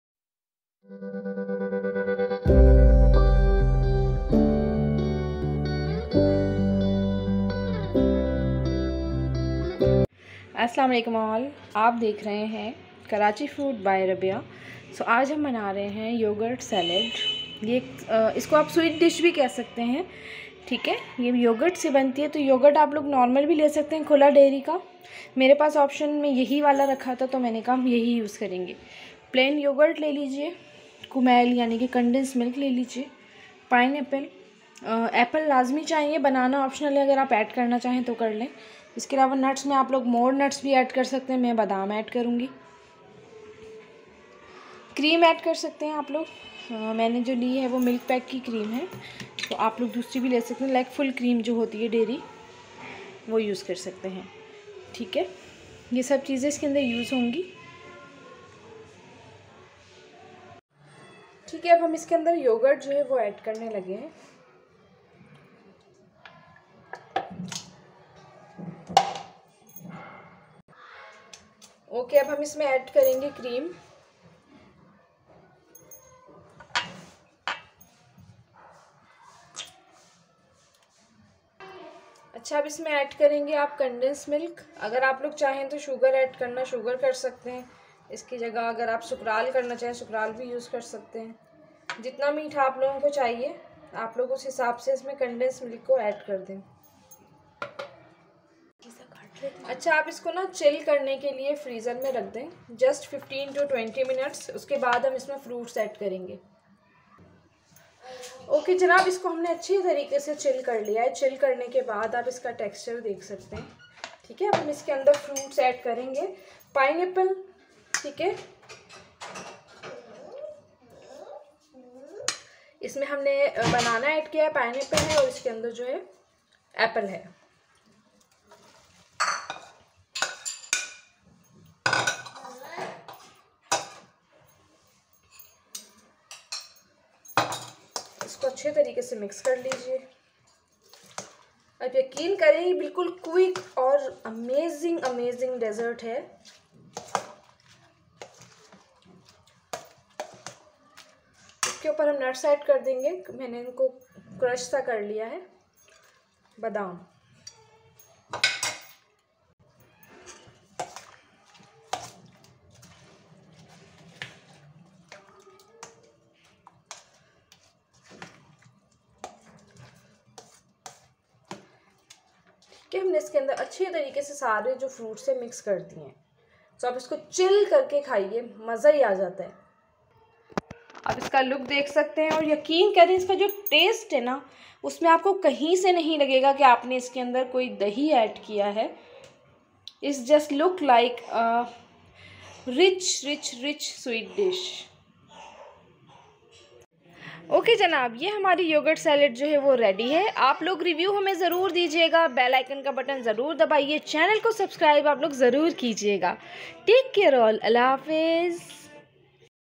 Delicious Yogurt Salad(quick Sweet Dish)دہی کا میٹھا @biyavlogsofficial
Delicious Yogurt Salad(quick Sweet Dish)دہی کا میٹھا

Ingredients 👇

Yogurt
cream
apple
banana
pineapple
condensed milk
Almond